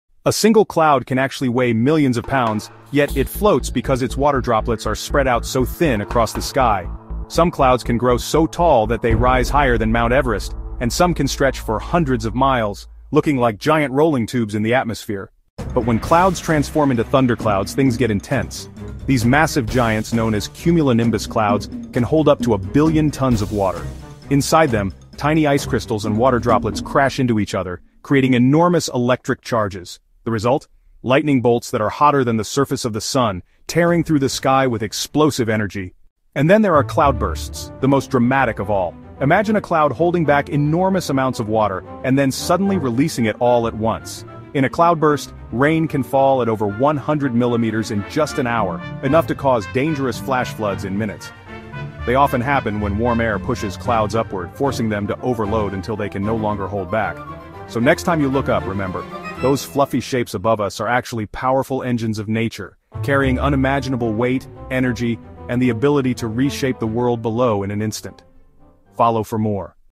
You Won’t Believe How Much a Cloud Weight
Clouds aren’t as innocent as they look… some weigh millions of pounds, create lightning hotter than the Sun, and can unleash deadly floods in minutes. Nature is way scarier than we think. 🌩️⚡